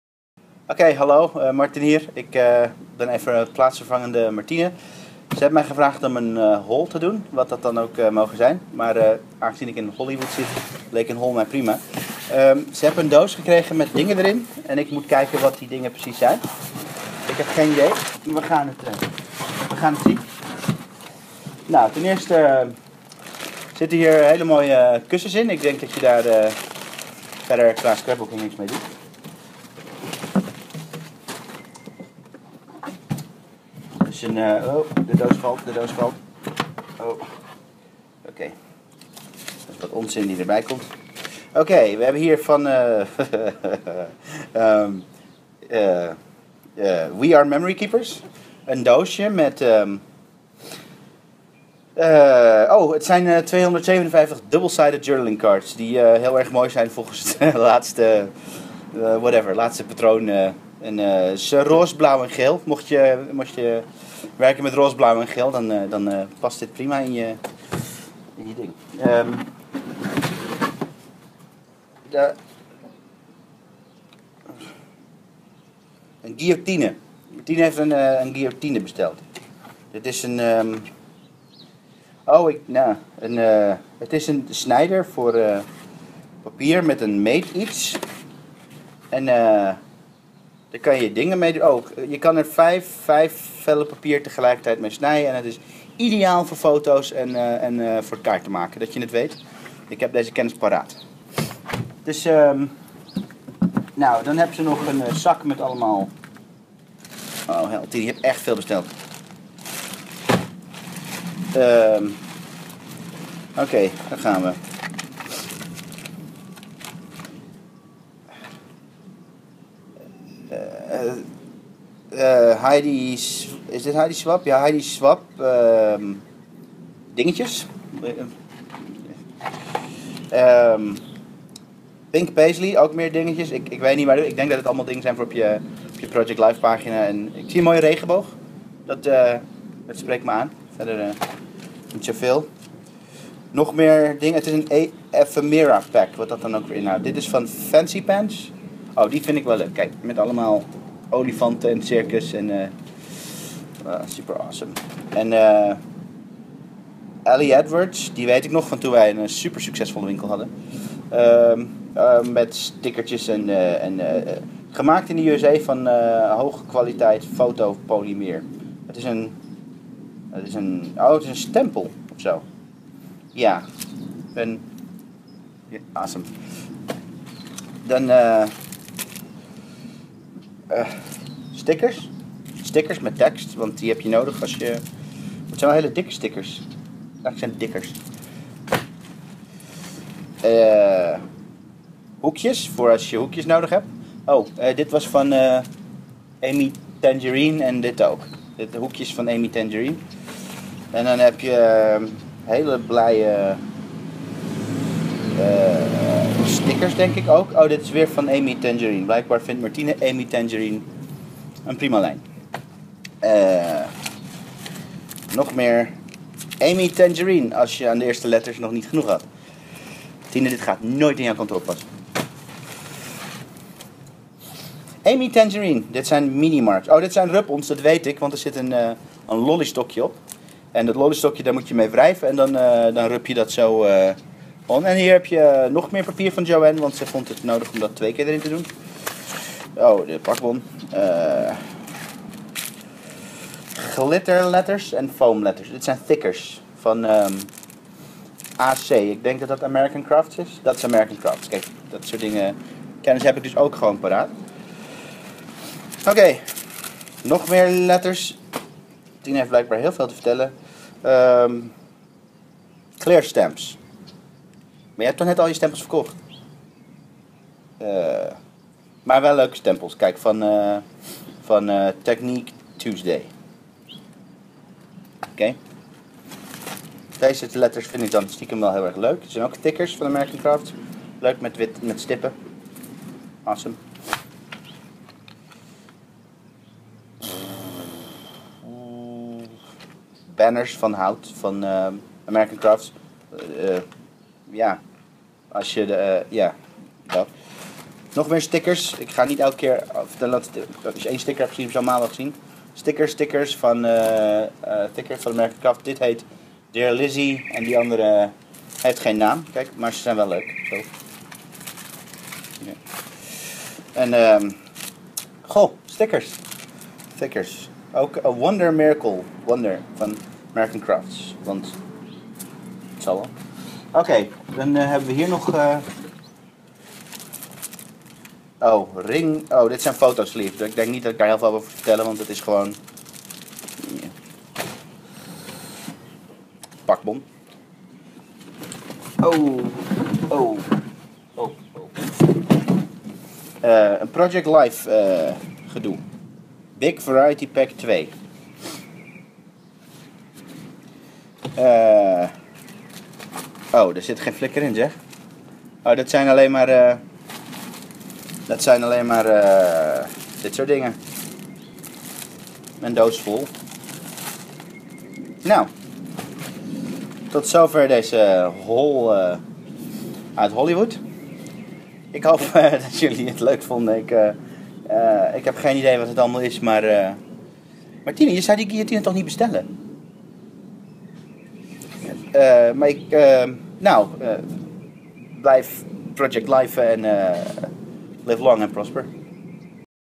Jo-Ann Haul door Martin
Tja, dan bestelt Martine een doos spulletjes in Amerika, maar dan is er zelf niet om een Haul te doen. Ik neem die taak dan met liefde van haar over, want mijn kennis van de hobby-industrie is uiteraard ongeëvenaard. Geniet van deze - eh - 'unieke'  Haul.